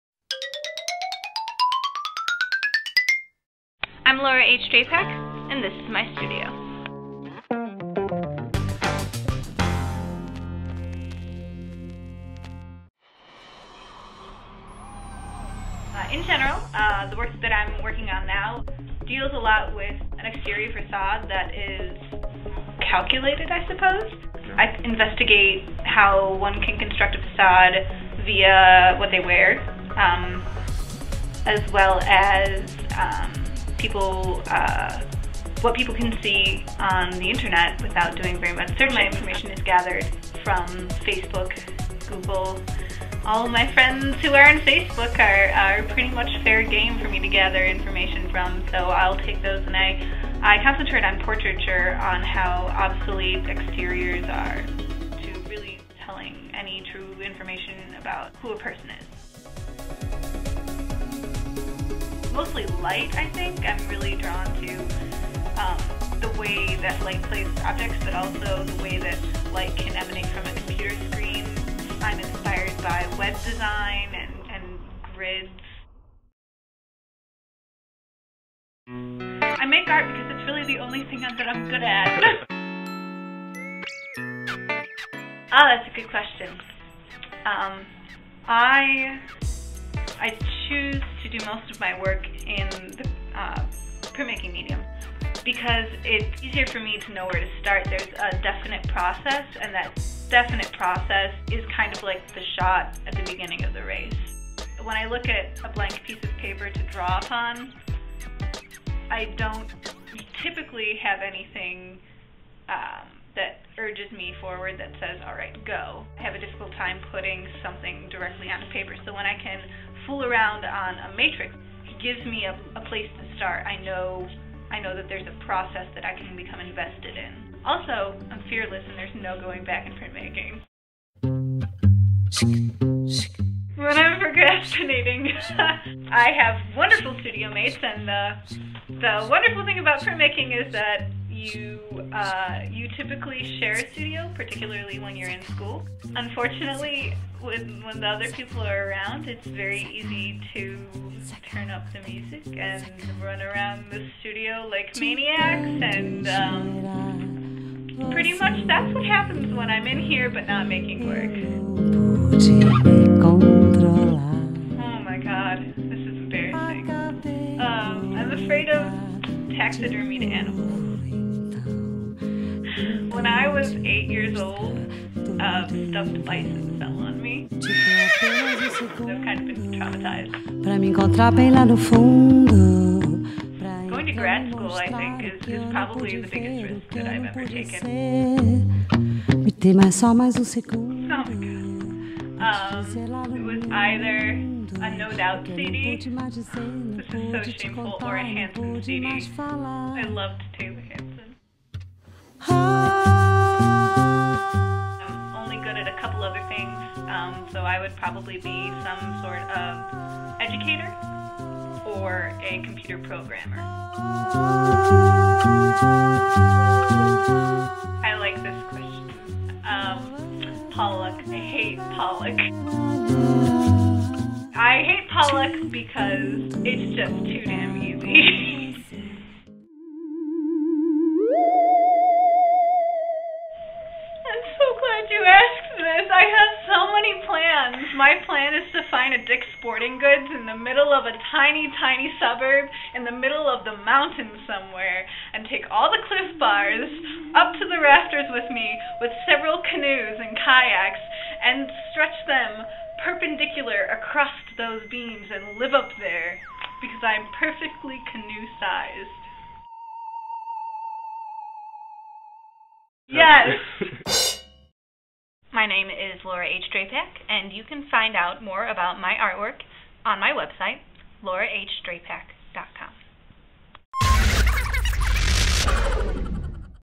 IN THE STUDIO: Laura H Drapac
I visit the studio with Laura H. Drapac, and printmaker working in Denton, Texas. For more information on Laura, go to her